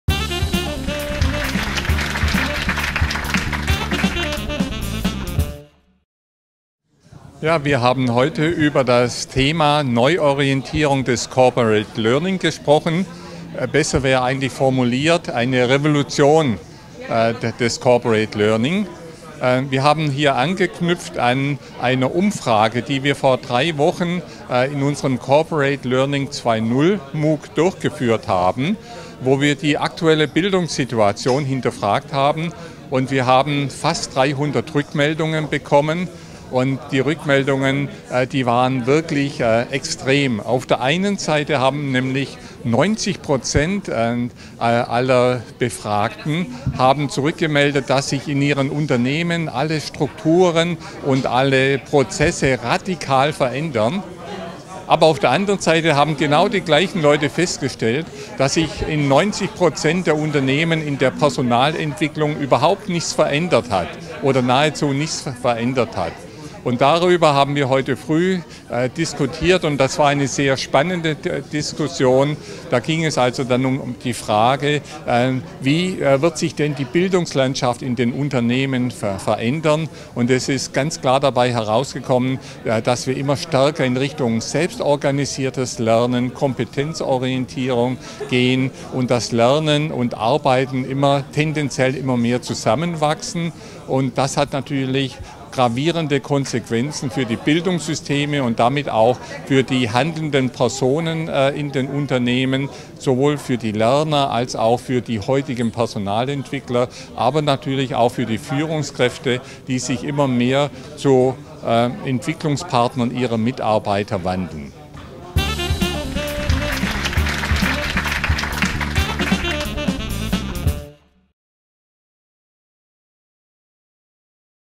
CLC15-Interview mit Prof. Werner Sauter
Am Rande des CorporateLearningCamp 2015 (CLC15) zeichnete ich eine Reihe von Interviews mit Sessiongebern auf. Hier das Interview mit André Lenich (Mediendidaktiker) und Jochen Werne (Bankhaus August Lenz).